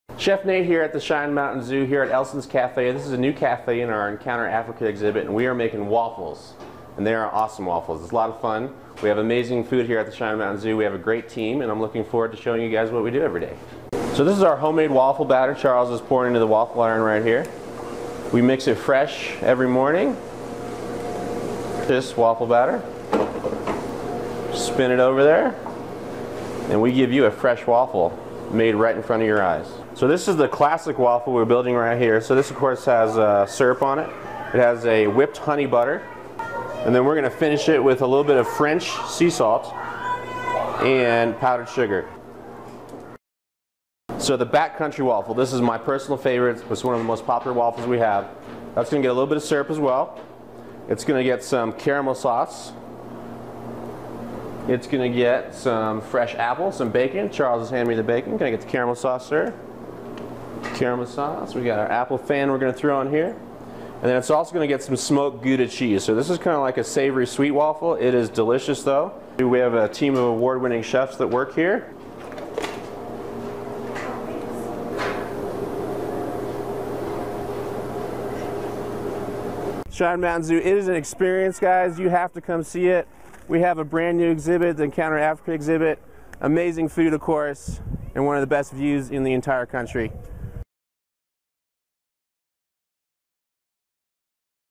Cheyenne Mountain Zoo's Waffle Cakes at Elson's Place Cafe
Chef Nate Dirnberger of Cheyenne Mountain Zoo shows off the newest cafe, Elson's Place, and the delicious treat he calls a waffle cake.